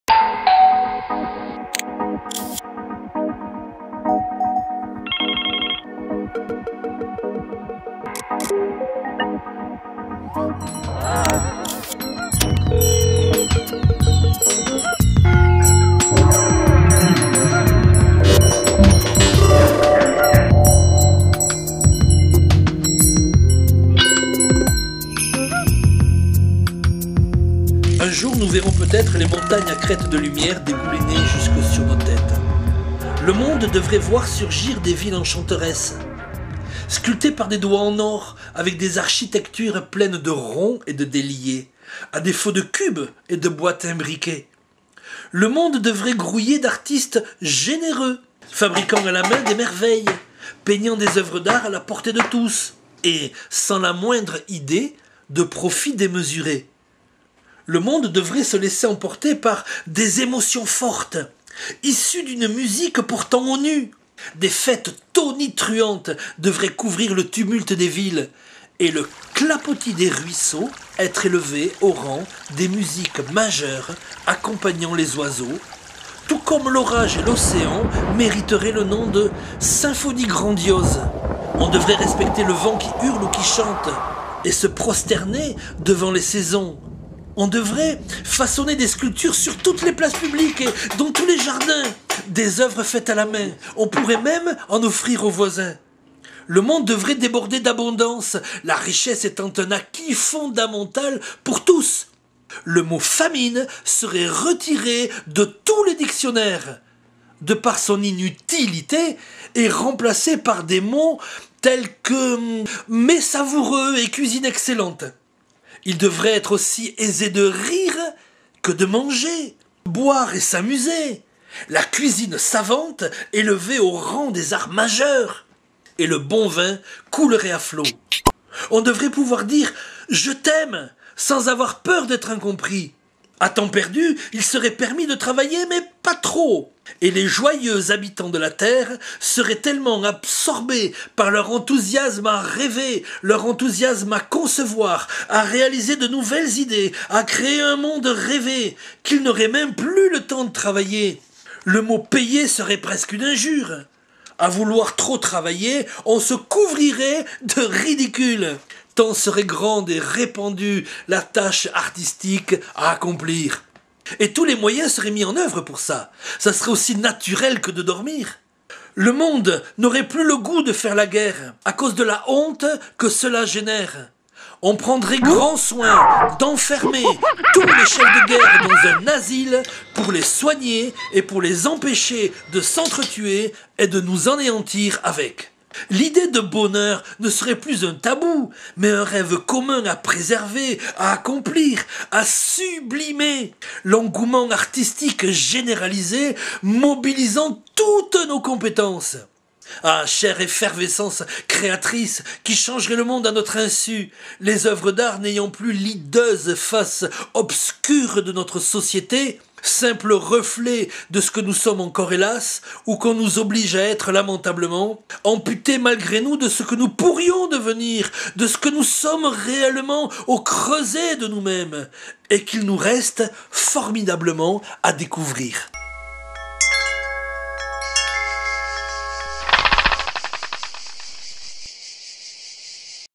villes enchanteresses
Villes enchanteresses, extrait du livre "Le voyage éphémère" de Lou Florian